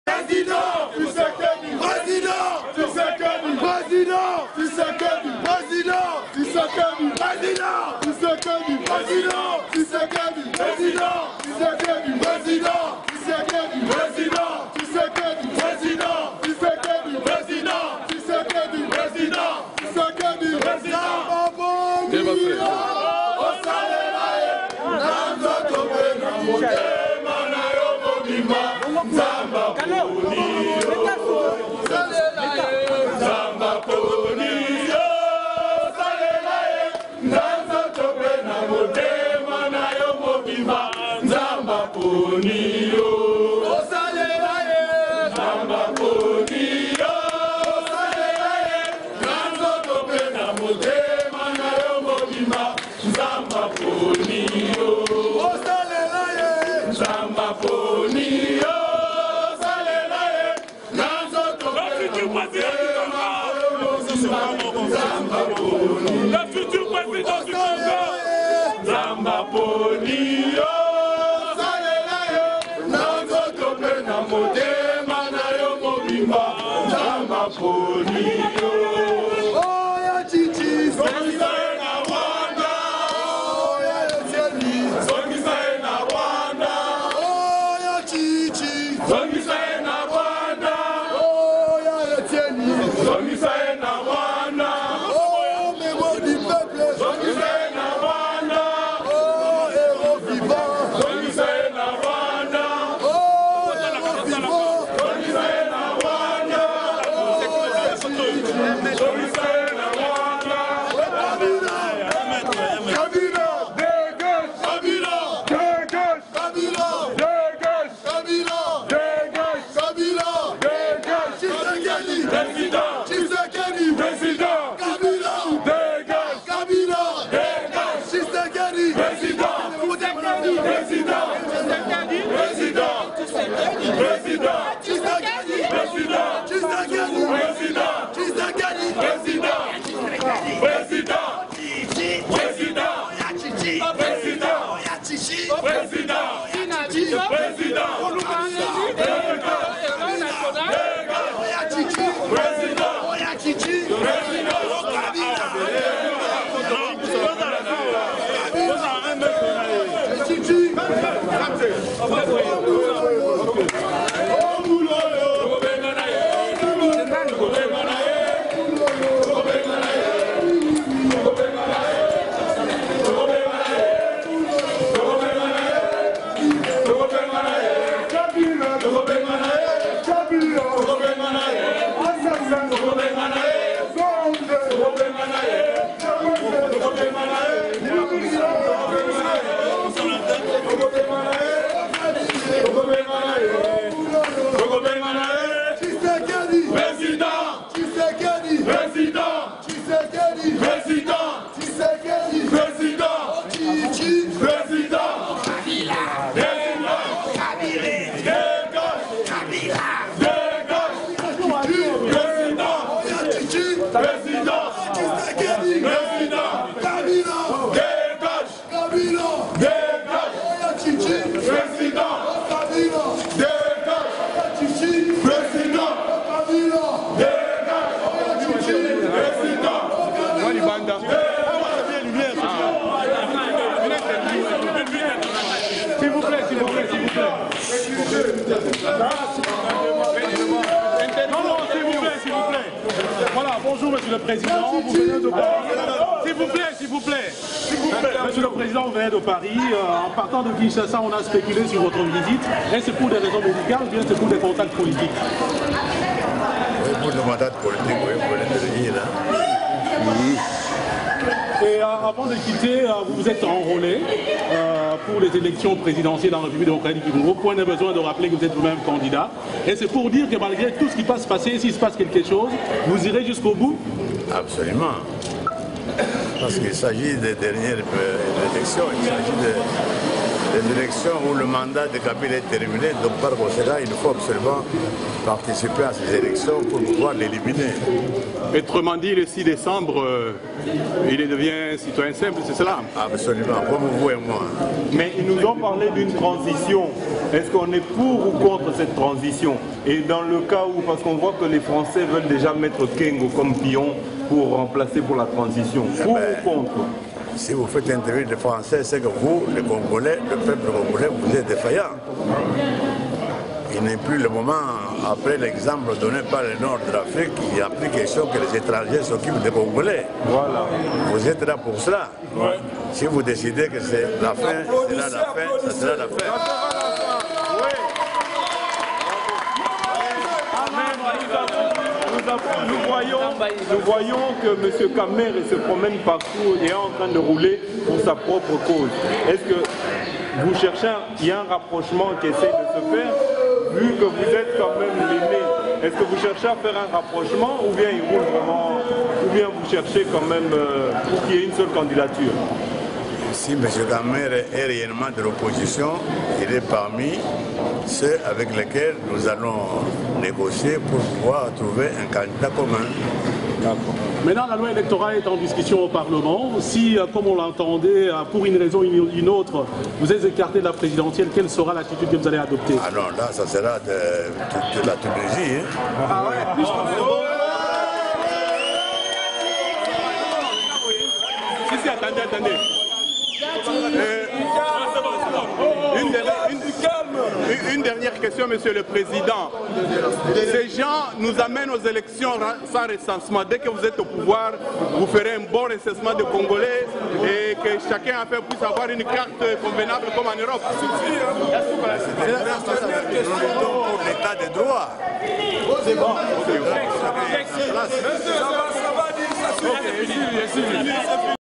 TSHISEKEDI ACCUEILLI A LA GARE DU MIDI DE BRUXELLES PAR DES CONGOLAIS
Le Blog de Cheik Fita présente : TSHISEKEDI ACCUEILLI A LA GARE DU MIDI DE BRUXELLES PAR DES CONGOLAIS